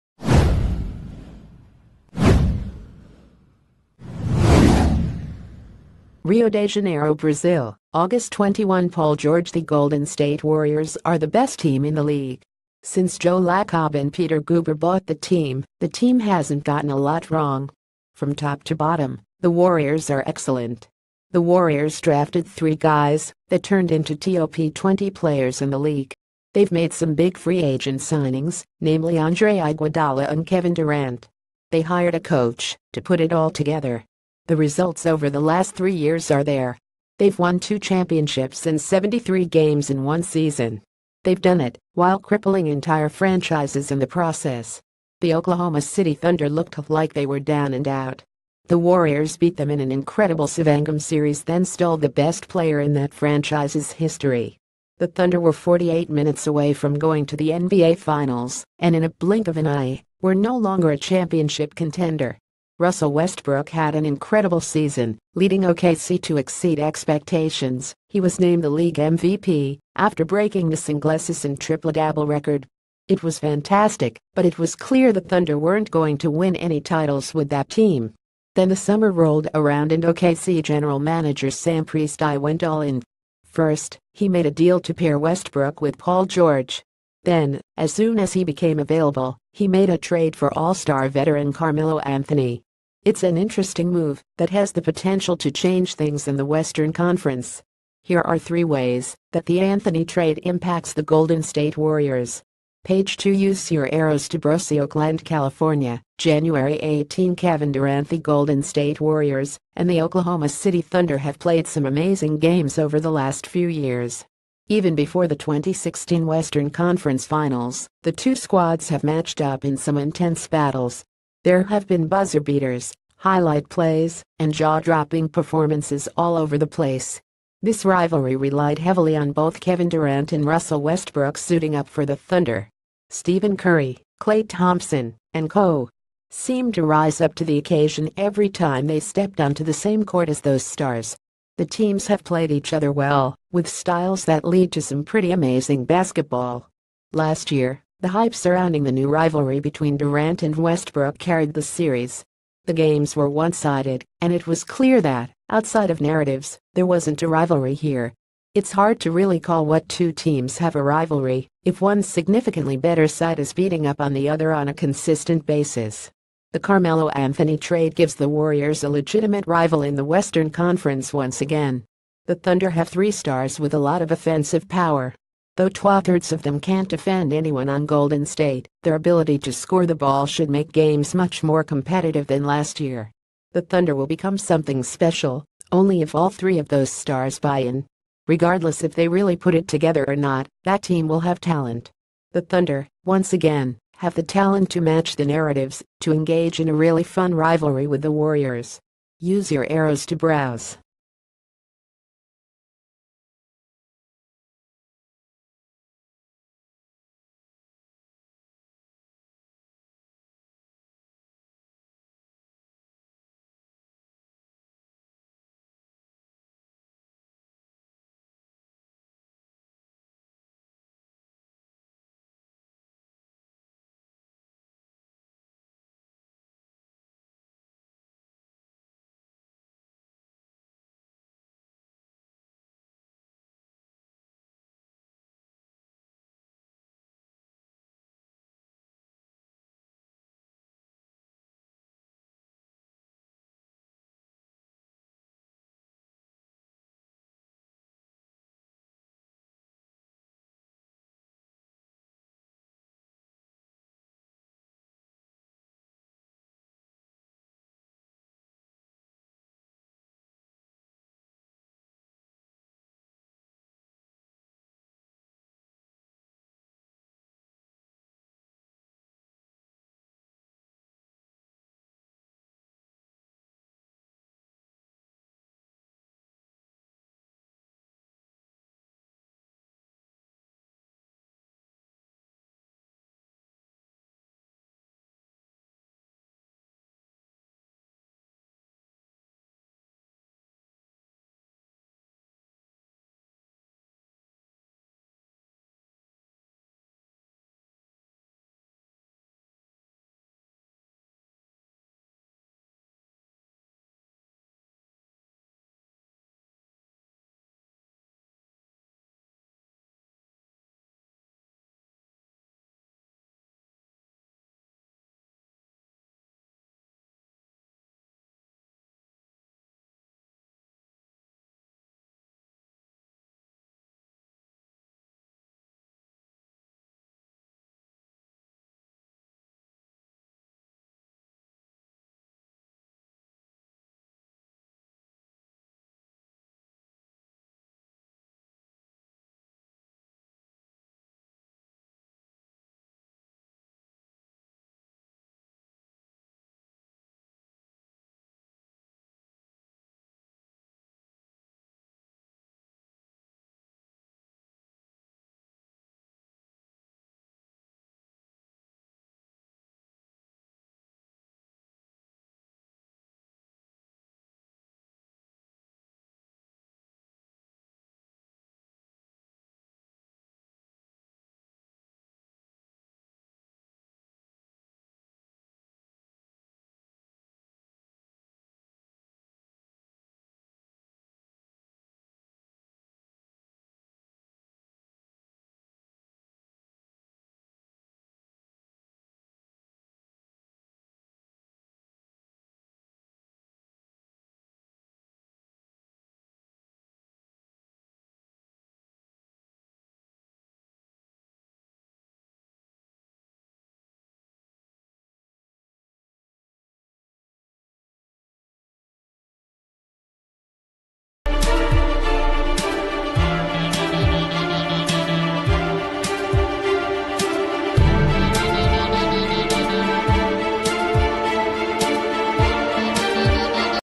Breaking News | Golden state warriors: three ways carmelo anthony and paul george trades impact dubs
Golden state warriors: three ways carmelo anthony and paul george trades impact dubs

RIO DE JANEIRO, BRAZIL – AUGUST 21 Paul GeorgeThe Golden State Warriors are the best team in the lea...